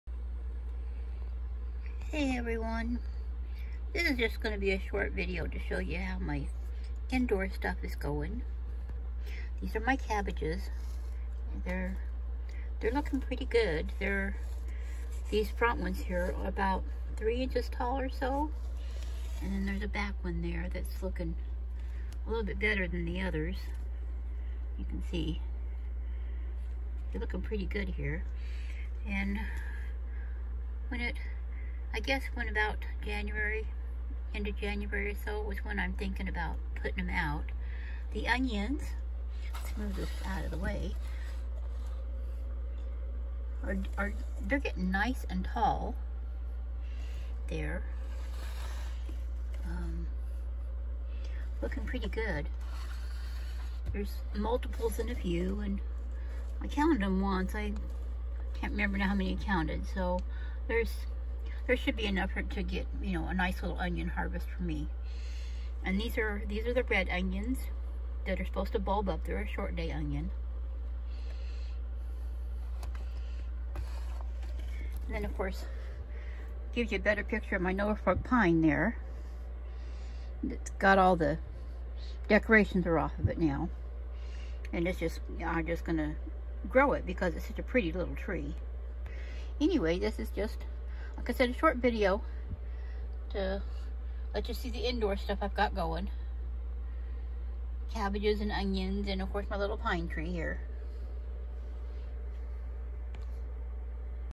End of December Indoor Gardening
A short video of indoor seedlings.

Onions are Red Creole and the cabbage are Early Round Dutch. Both from Hoss Tools.